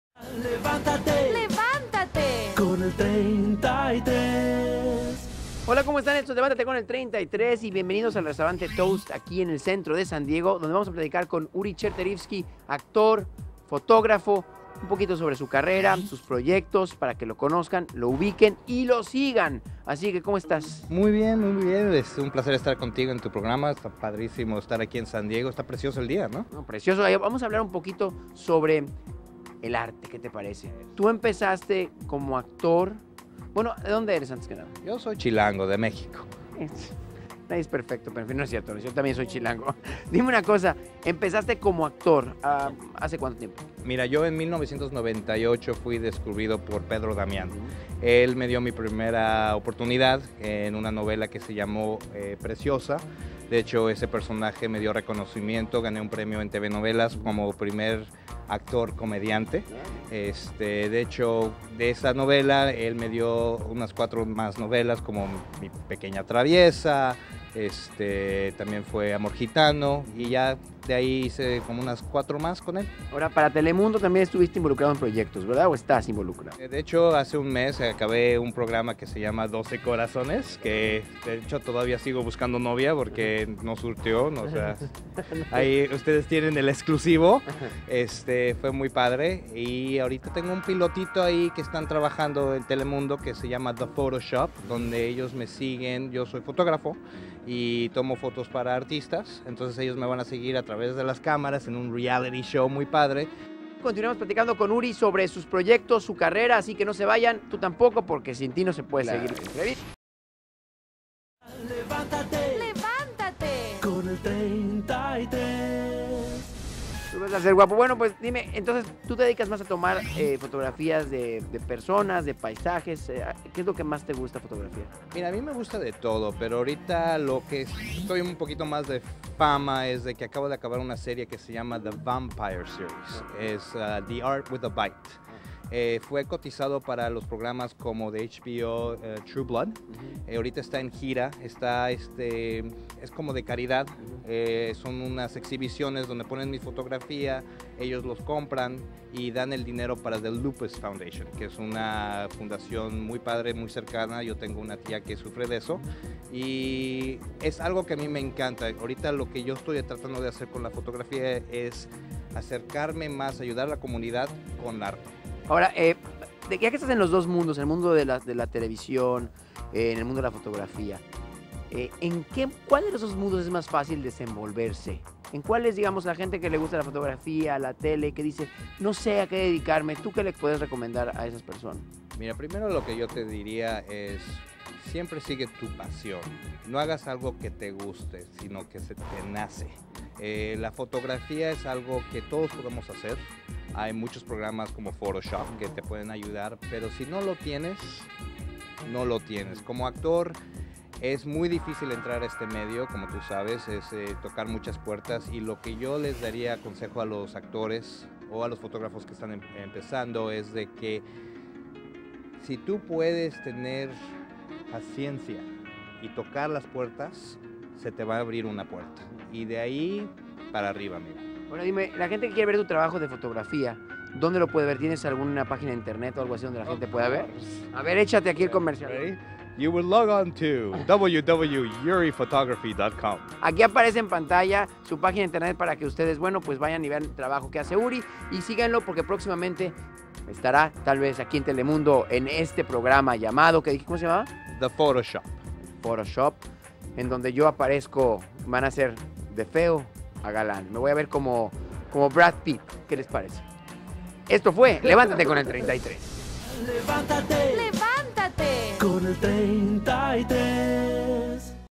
Telemundo Entrevista con Uri Chartarifsky
Conoce al gran actor mexican Uri Chartarifsky, Y siguelo en su carrera en Telemundo